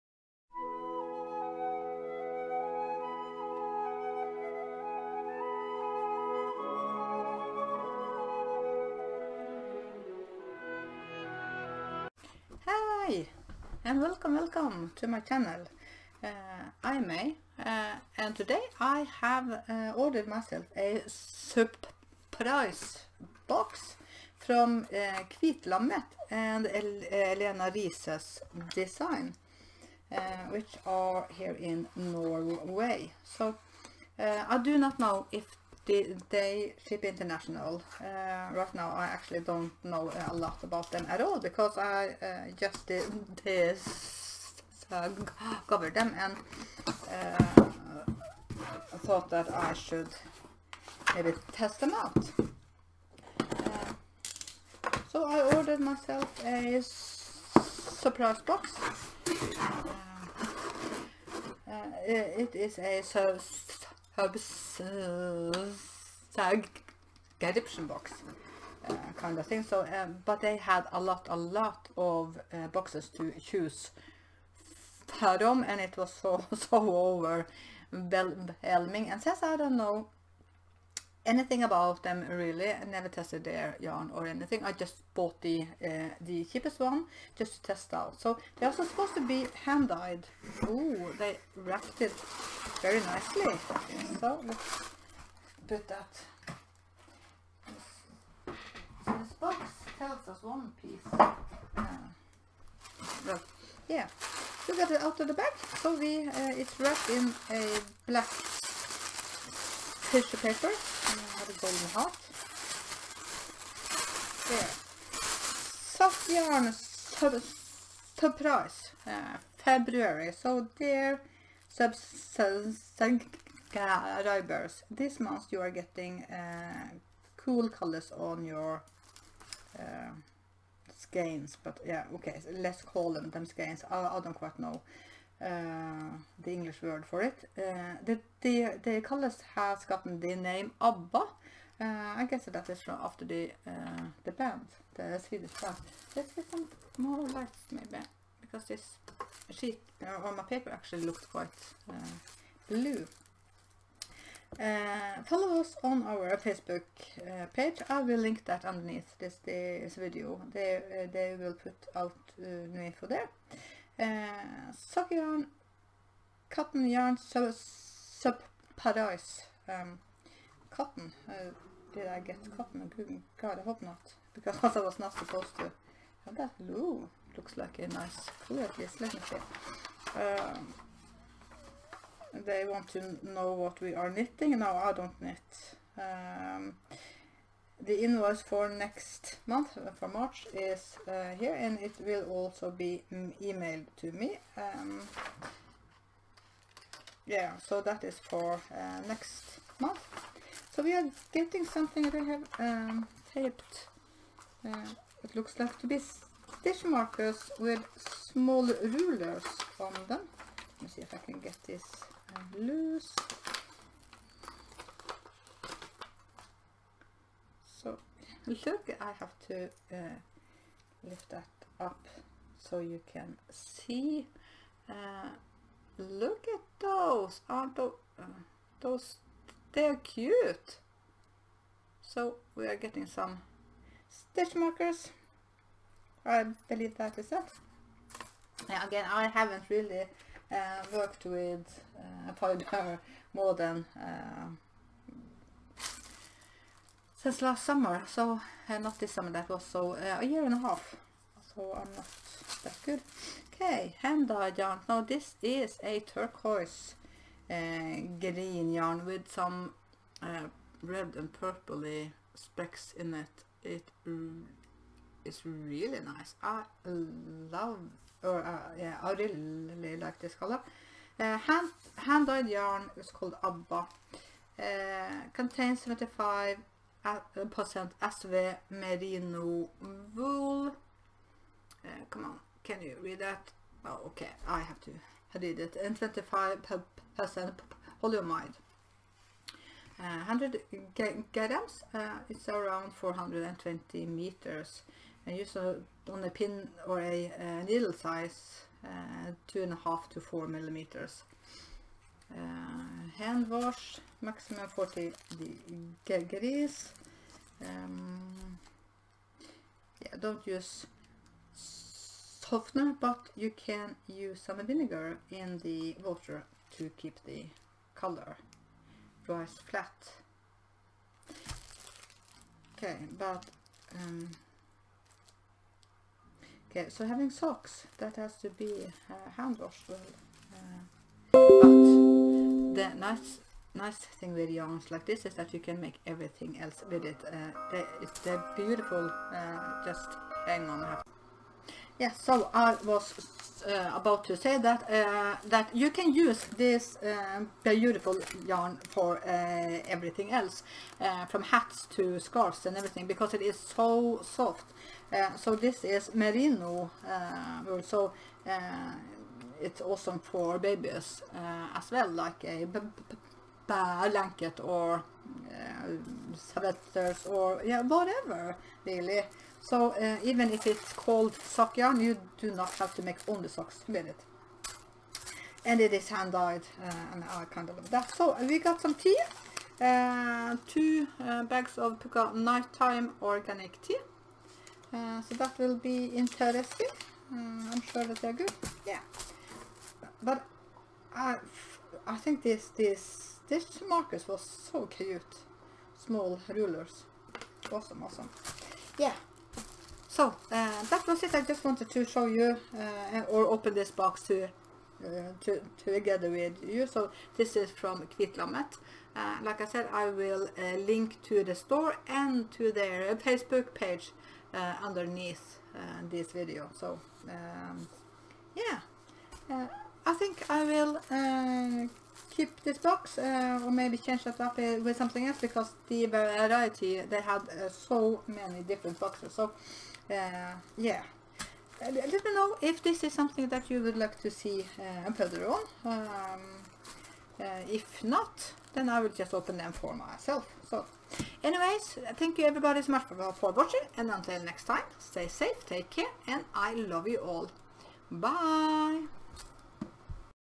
Surprise-box Hand dyed sock yarn from Kvitlammet
I got a surprise-box from Kvitlammet.

Sorry for the old nail polish.

The Amazon UK list